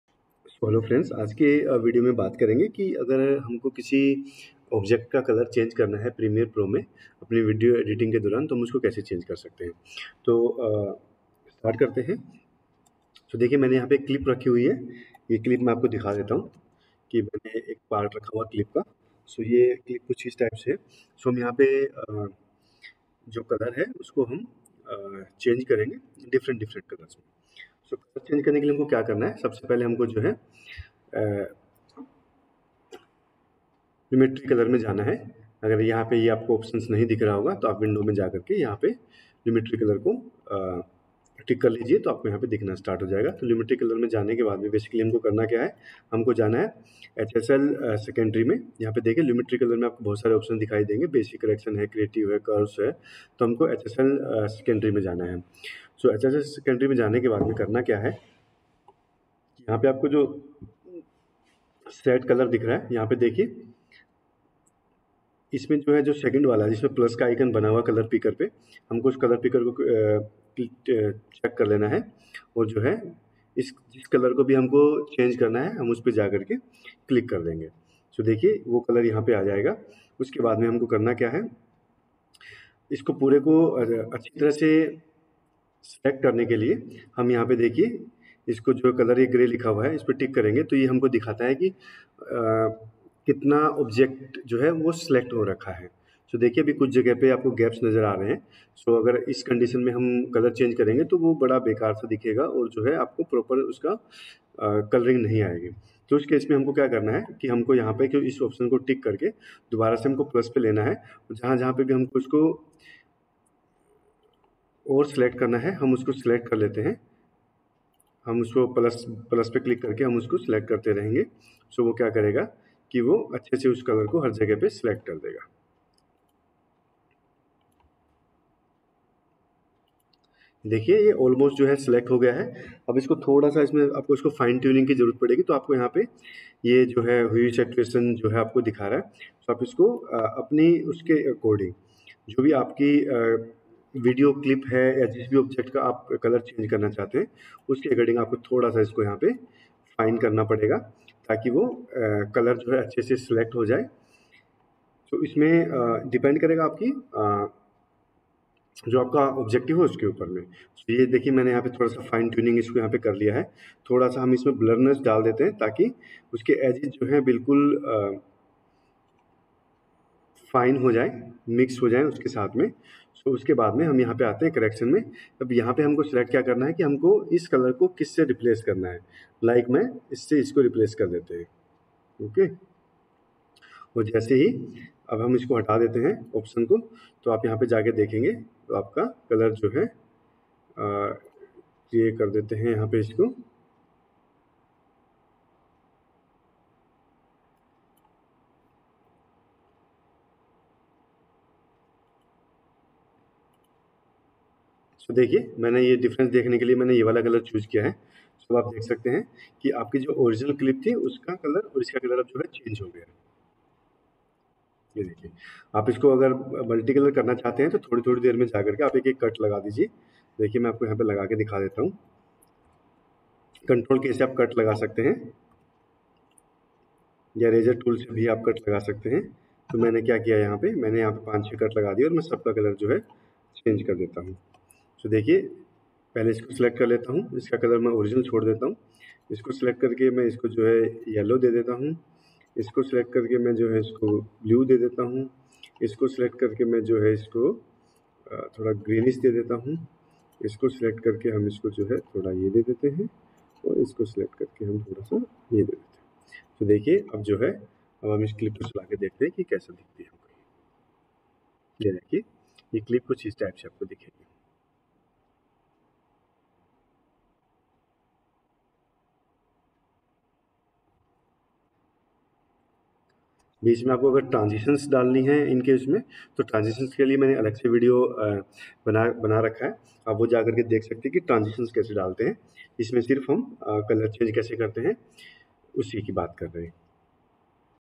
Adobe Premiere Pro – Change Color of an Object
How can you change the color of an object in your video using HSL Secondary tool